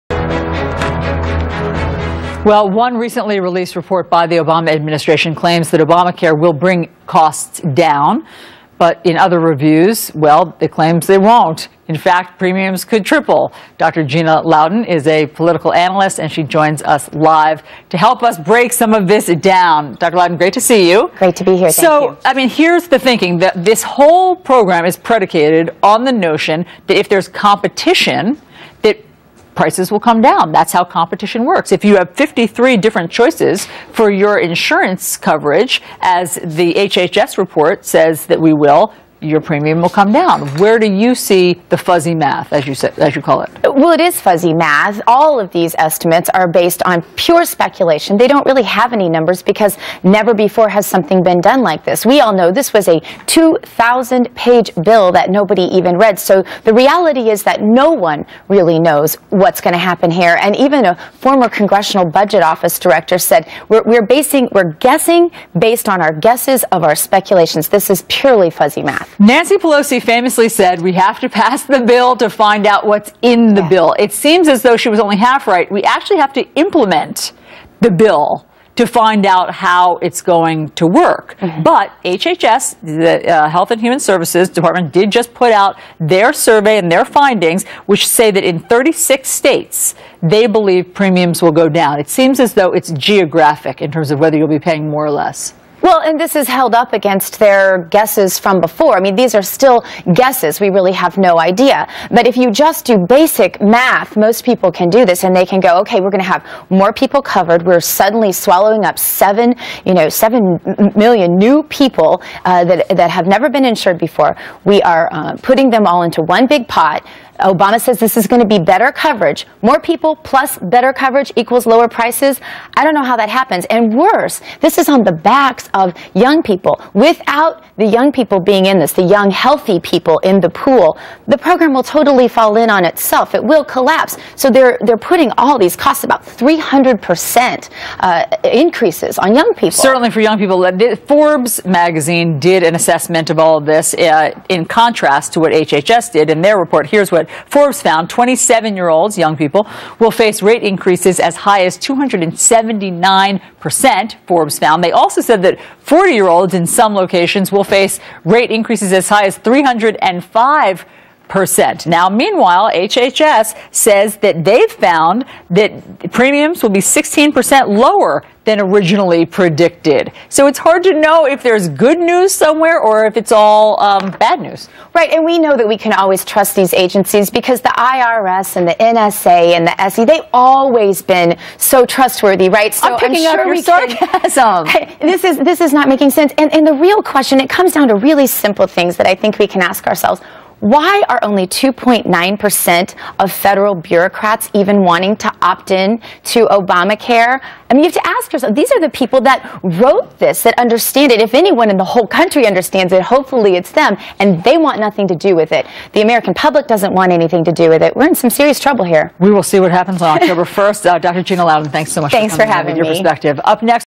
Obamacare premiums will increase by 300% or more for 30 to 40 year olds
Under Obamacare, premiums for 30 to 40 years will rise by by 300% or more in many states. Obamacare is being supported on the backs of young people and young people won't be able to afford these huge increases. If they don't sign up for Obamacare, the program will collapse. HHS on the other hand said premiums will be lower by 68% than previously predicted. But how can we really r trust the HHS given the IRS and NSA. If Obamacare is so damn great why is the Administration making such a major push to sell it.